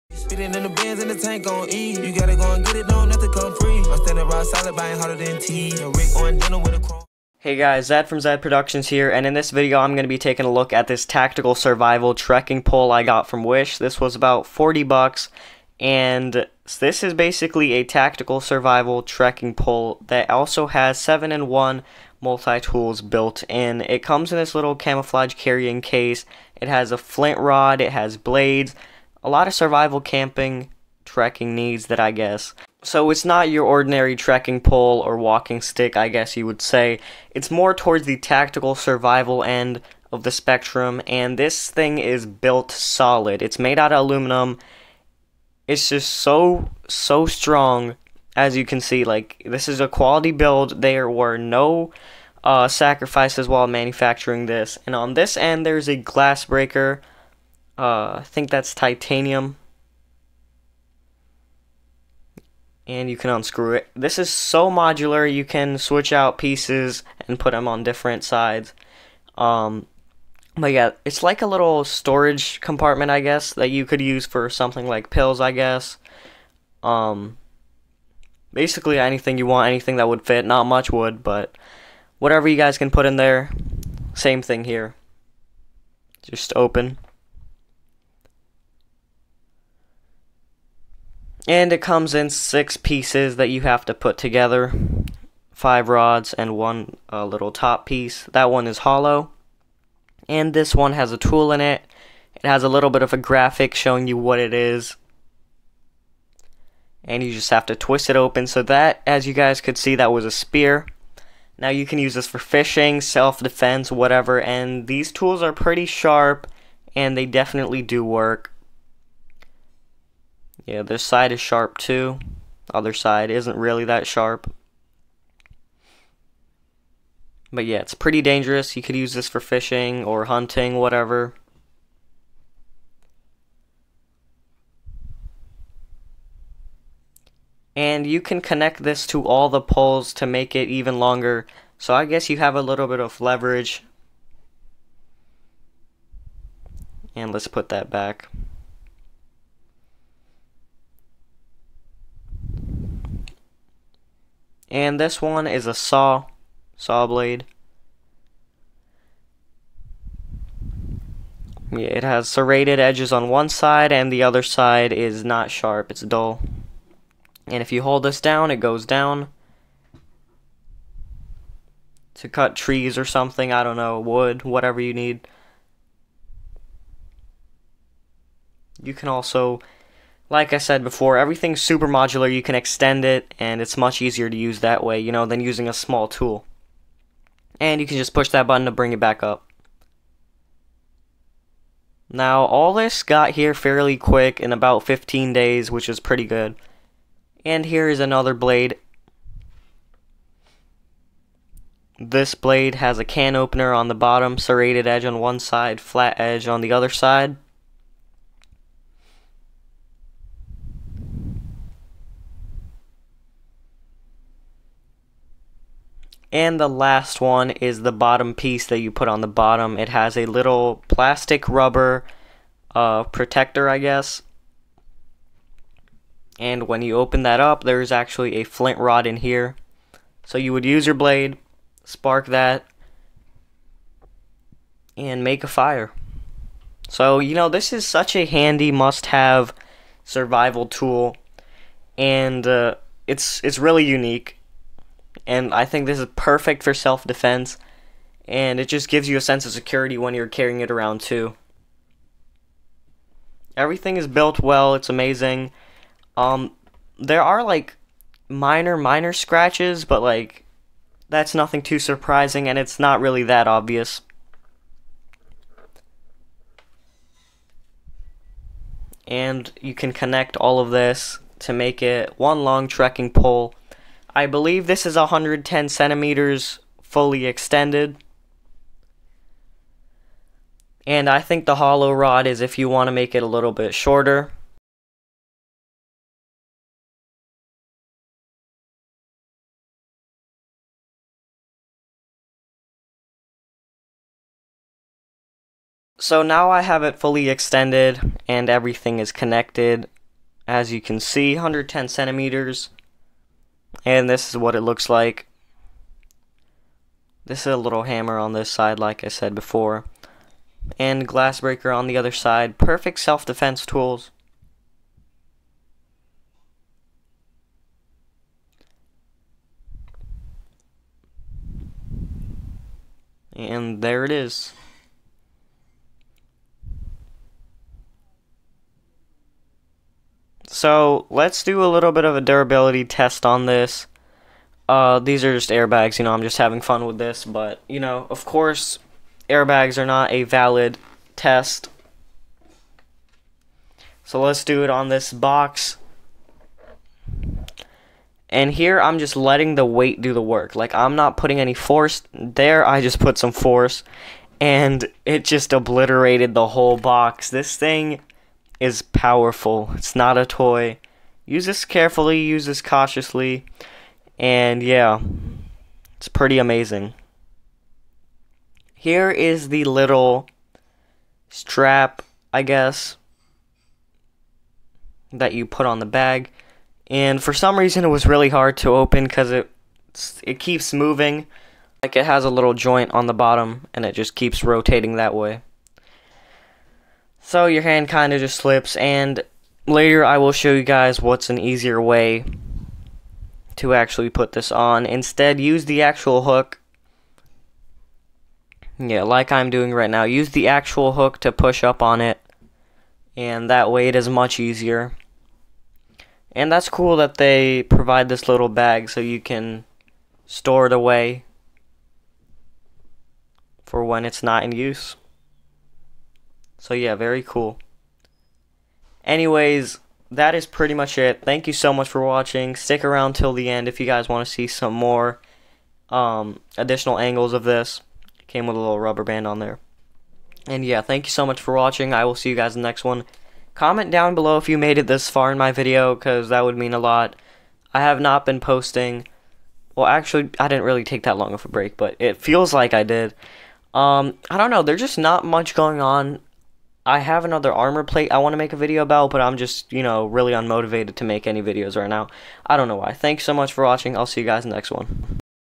Tactical Survival Trekking Pole 7 in 1 Multi-tool from Wish.
Link To Purchase:💰 Look what I found on Wish! I'm giving you $5 off your first purchase with code: nrhccrt

Tactical tool for hunting, camping, and self defense from wish